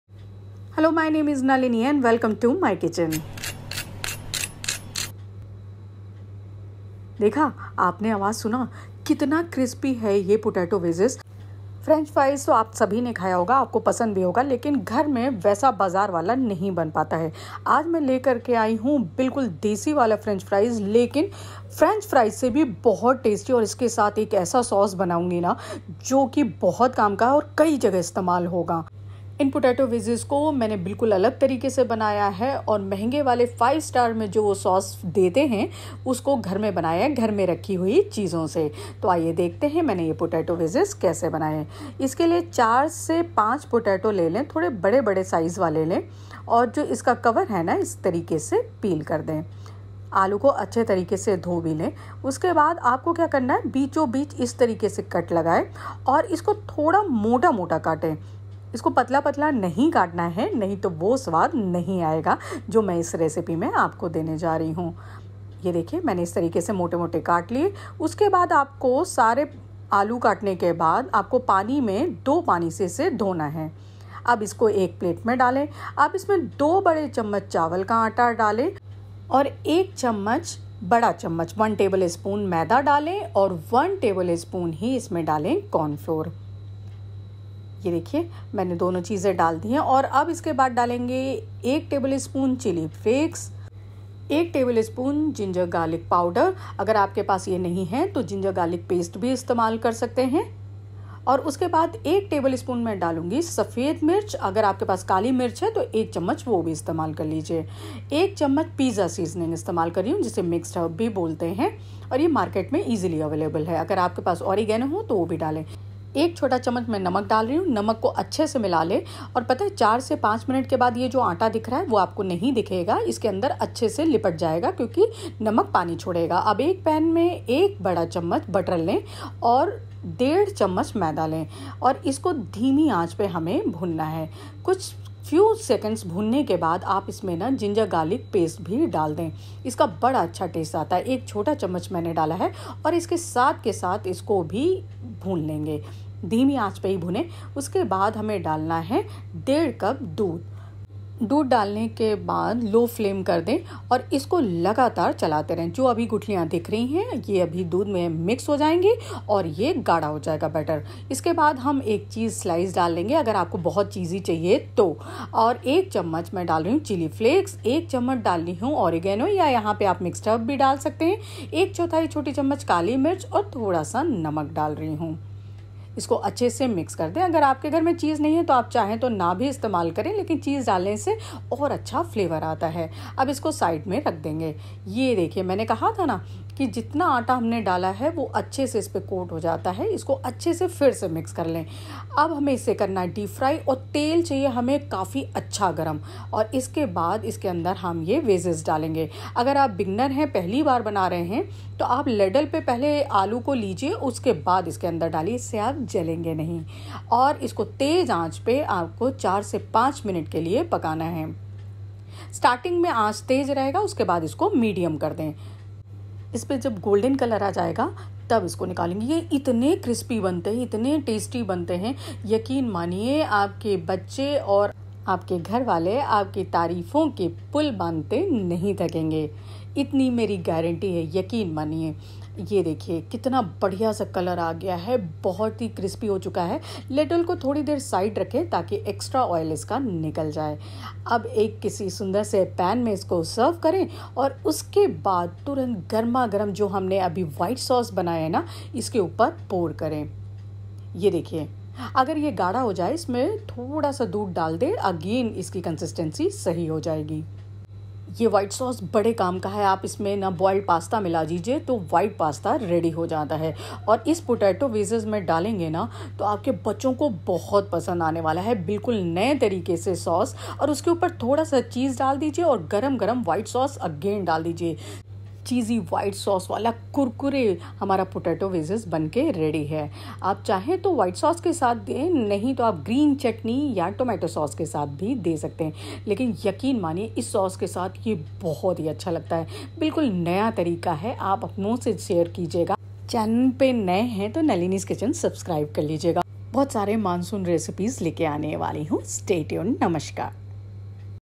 रिमझिम बारिश मे बनाइए आलू का ऐसा नया नाश्ता जिसे खाकर बारिश का मजा दुगना हो जाए Monsoon Special 🌧️
Hey Guys To Barish Ka Mosam chaya hua hai Aur Naya Naya kuch khane ka Mann Bhi Hota Hai To isliye Nalini Layi Hai Aapke Liye Moonsoon Special Recipe so Recipe Dekhiye And Use Apne friends Family Ke Saath Share Zaroor Kare And Acchi lge To Subscribe Zaroor Kare ❤️🙏